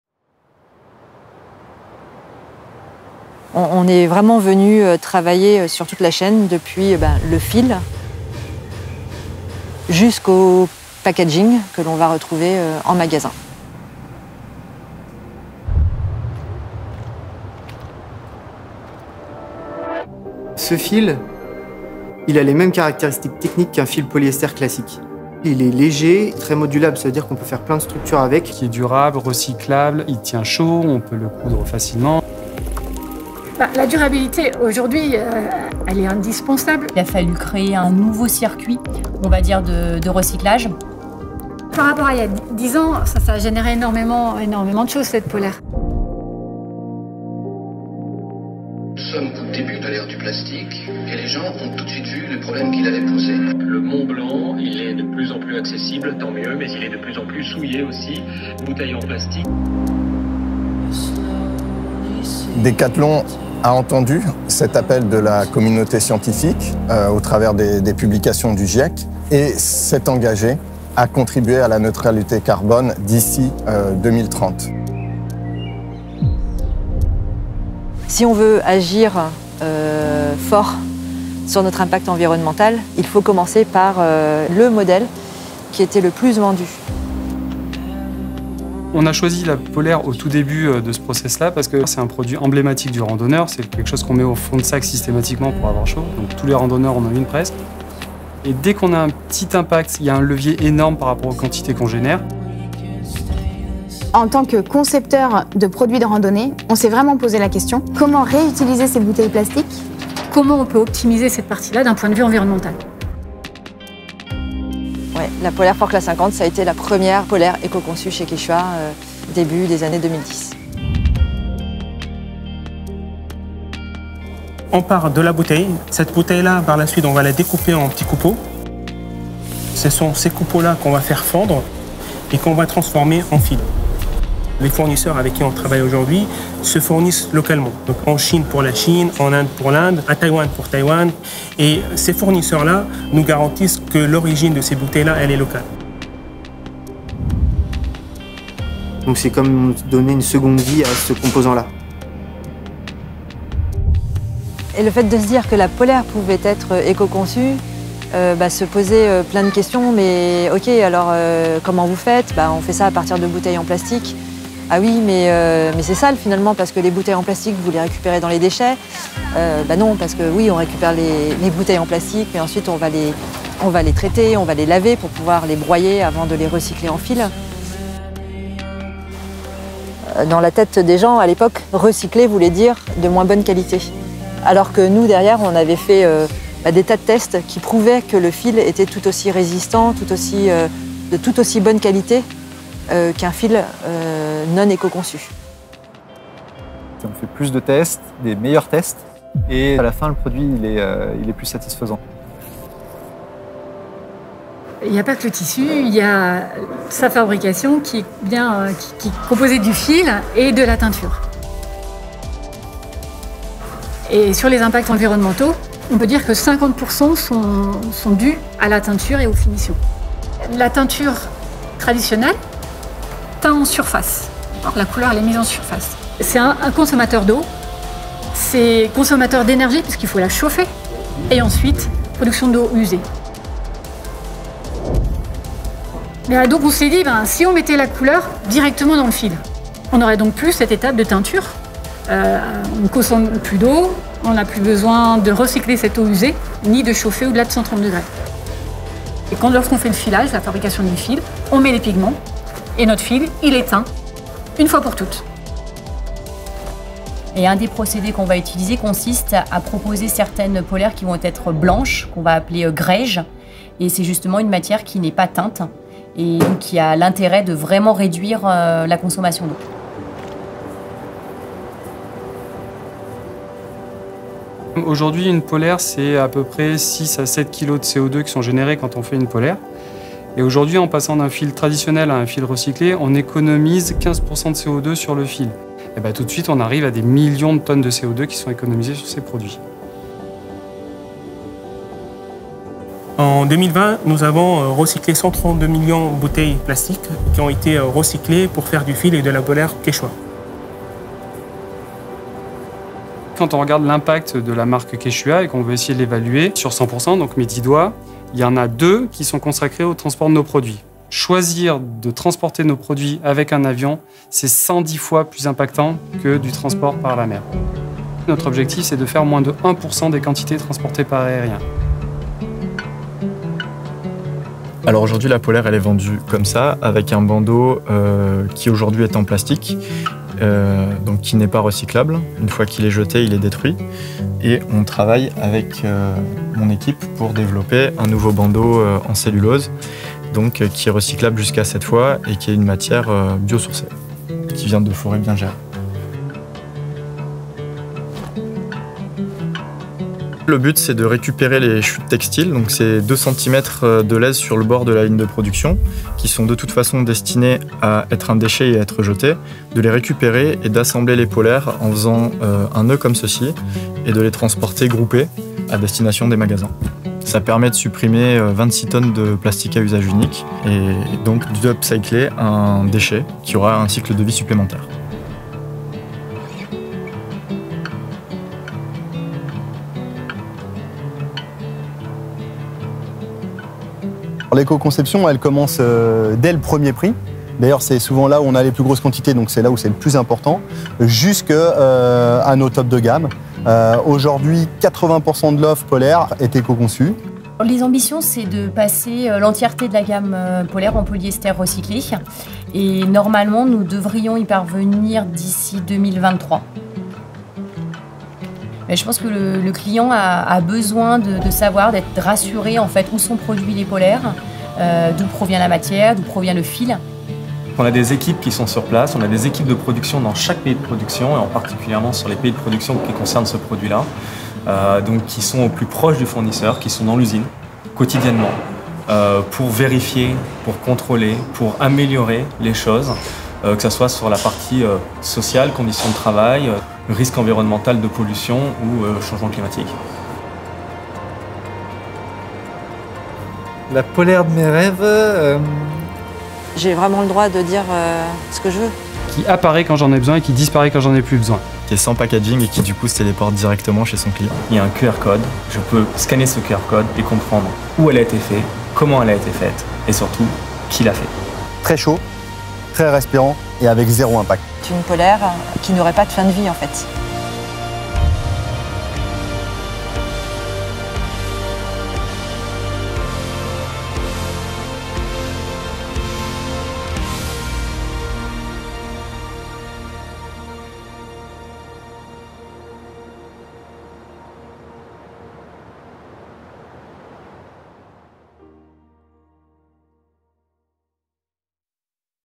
Pull the thread 🧵
This year marks our 10th birthday of recycled fleeces!
For 10 years, we’ve been striving to reduce our impact on this iconic product. Since 1987 and our 1st fleece, we’ve been making trendy, sporty fleeces for all our customers! In 2010, there was an environmental shift with the first eco-designed Quechua fleece: the Forclaz 50. That was just a start...